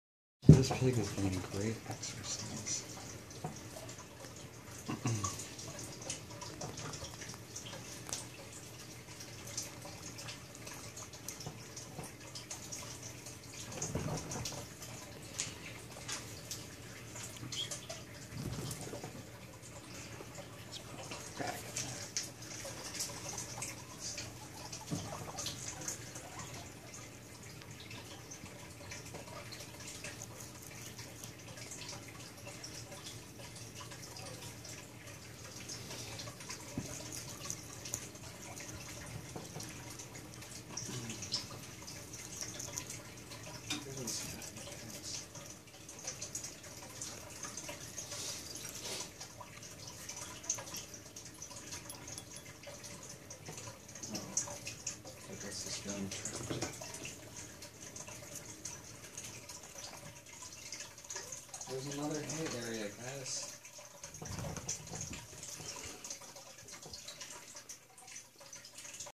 Hairless Guinea Pigs and their Living World house
Skinny pigs loving their new hay house!